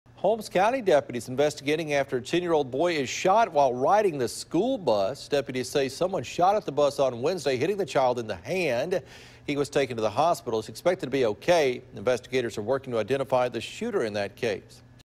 Holmes County student shot by stray bullet on bus
Student shot in hand while riding school bus